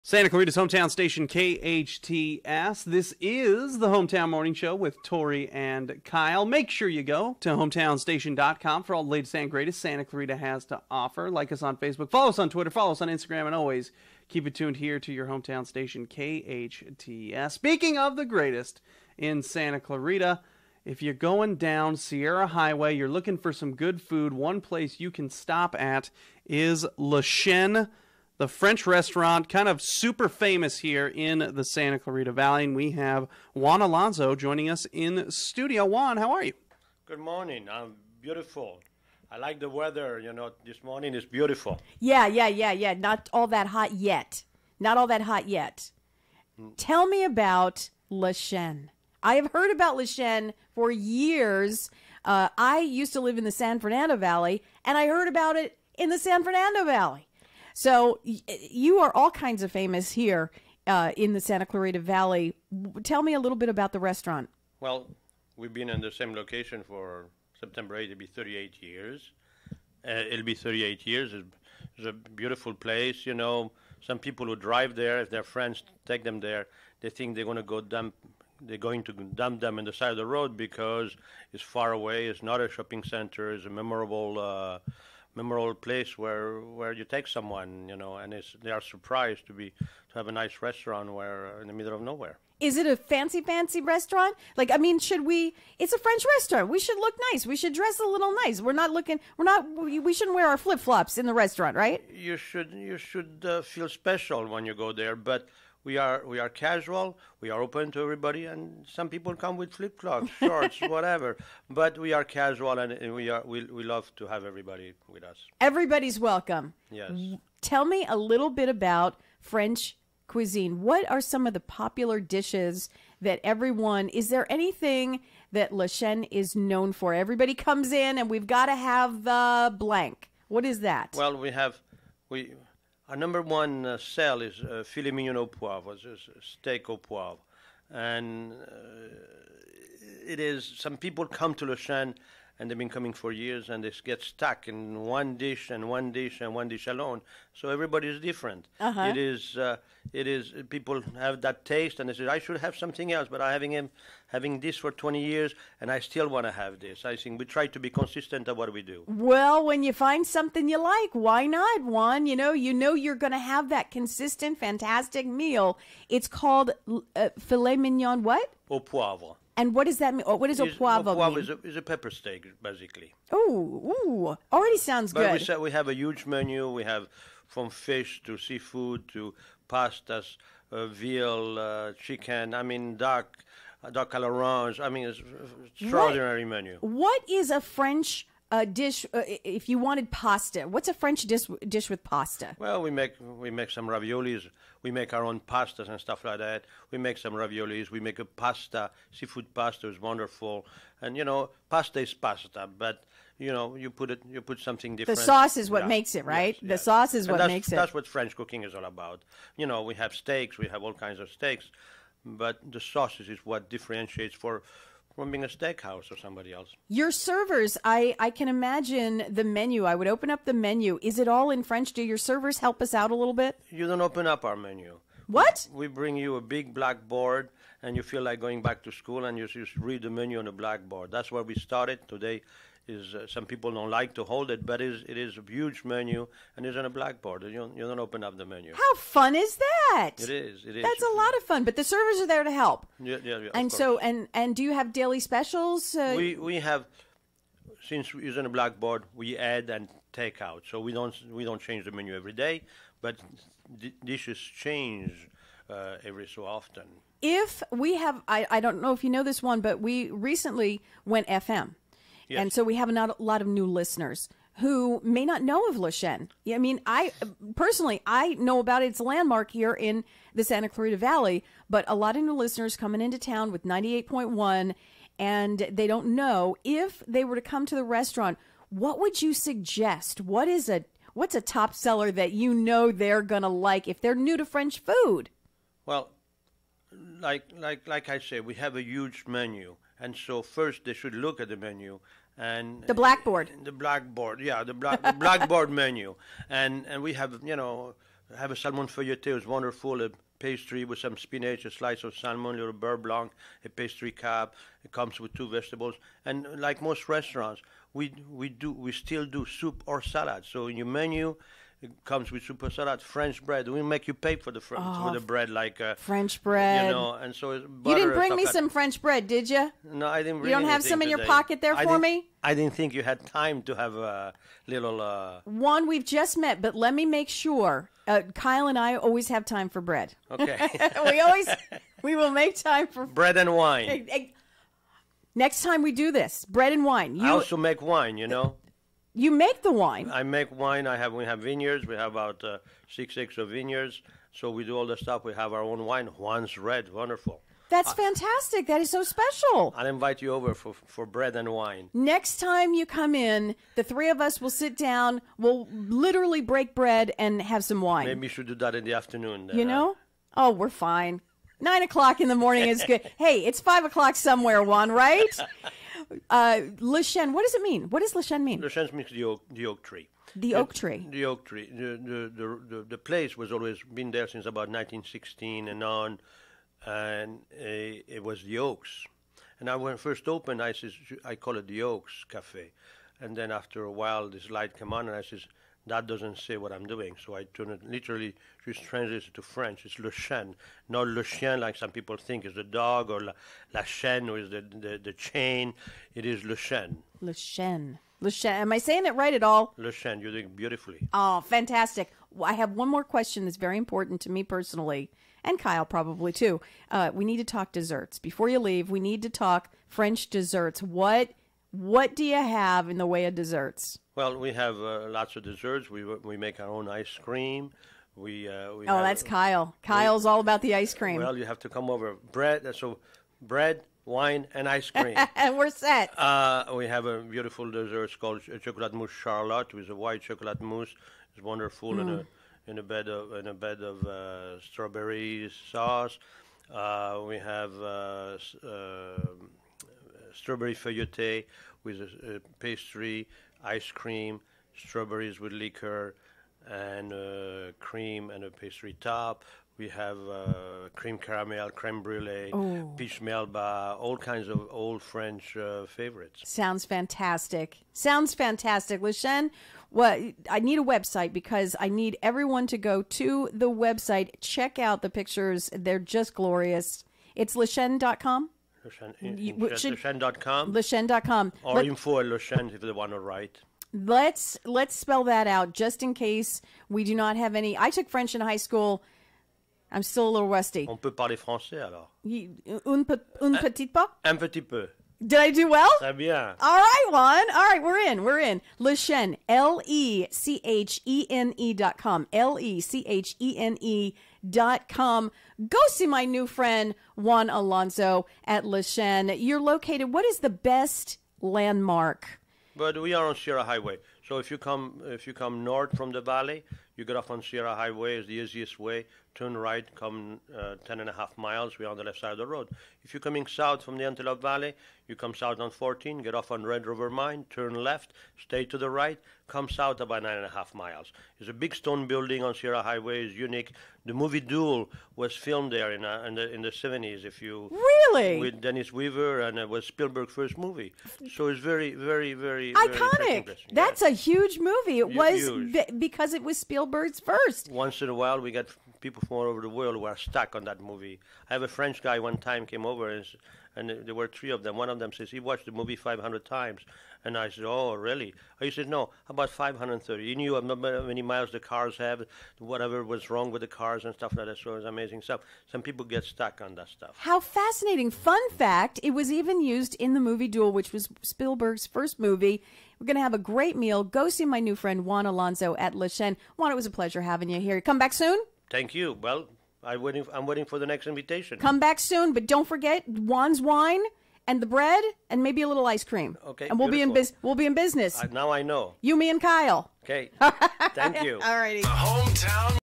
Juan Alonso From Le Chene - July 12, 2018 - KHTS - Santa Clarita
Le Chene French Cuisine Offers Fine Dining Experience To Santa Clarita Valley For Almost 40 Years

Le Chêne French Cuisine is a popular “destination” restaurant on Sierra Highway near Agua Dulce. The restaurant has been delighting customers with fine food and wines for 30 years.

The translation of the restaurant’s name “le chêne” means “the oak”.  This area has always been noted for its beautiful, centennial, oak trees. Starting in 1917, there has been a long tradition of naming businesses operating at this location after these magnificent oak trees.

The stone building where the Le Chêne restaurant is today has stood proudly for 87 years. The site was originally developed by the Dodrill family in the early part of the last century. William Arch Dodrill was born in 1888 in Stevens Creek, Lancaster County, Nebraska. His grandparents had emigrated from England and settled in Virginia. His grandfather was in the Revolutionary War, was captured, escaped, and changed his name from Dodridge to Dodrill in order to stay in America.